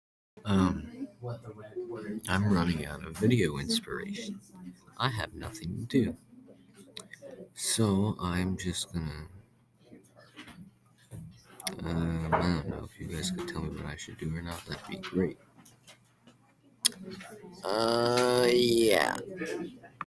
I have no video inspiration,if anyone can give me any
I need ideas to make videos,something that hasn't been done,leave your ideas in the comments please